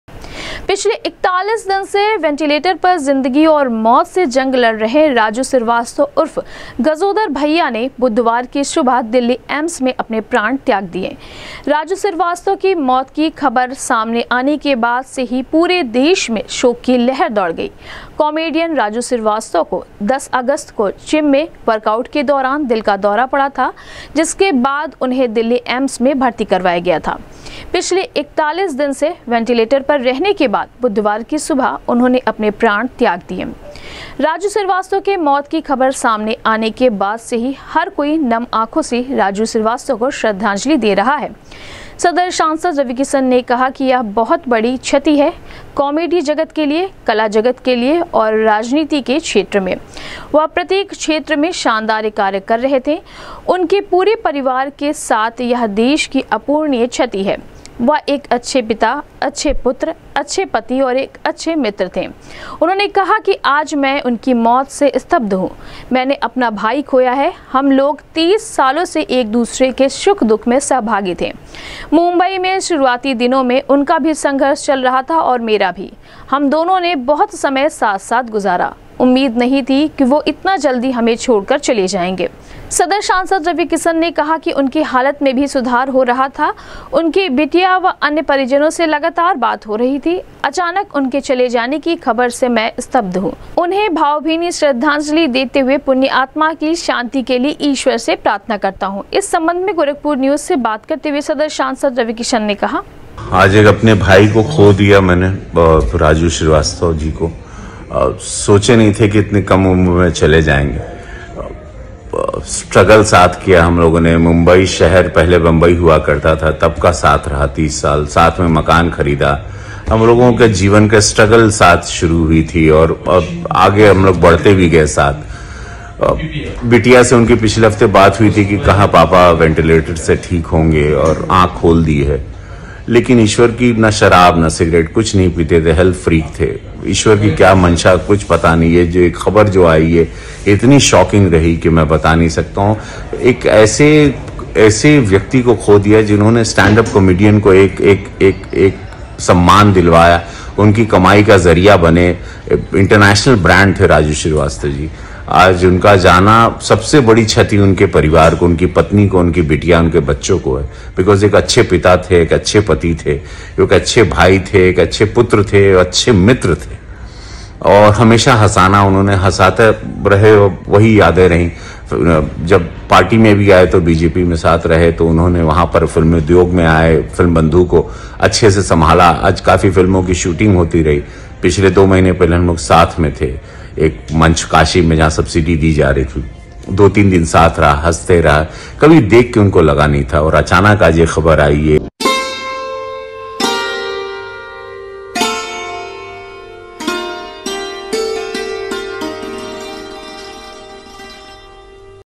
जिंदगी और मौत से जंग लड़ रहे कॉमेडियन राजू श्रीवास्तव उर्फ गजोधर भईया का बुधवार को दिल्ली एम्स में
जिंदगी और मौत से जंग लड़ रहे कॉमेडियन राजू श्रीवास्तव उर्फ गजोधर भईया का बुधवार को दिल्ली एम्स में हुआ निधन, 10 अगस्त को जिम में वर्कआउट दौरान पड़ा था दिल का दौरा, पूरे देश में है शोक की लहर, सदर सांसद रवि किशन ने कहा, उनकी मौत से हूं स्तब्ध,उन्हें विनम्र श्रद्धांजलि देते हुए पुण्य आत्मा की शांति के लिए करता हॅॅू ईश्वर से प्रार्थना